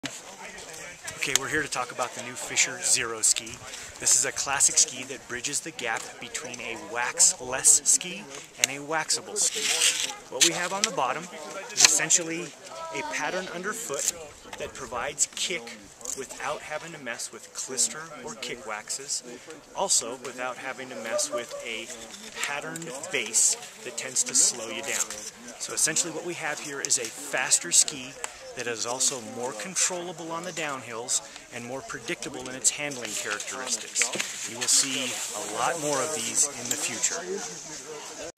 Fischer Zero Skis | Rubber Base Cross Country Skis |Waxless XC Skis
This video is review of Fischer Zero Skis, performance waxless skis that are fairly new to the nordic world.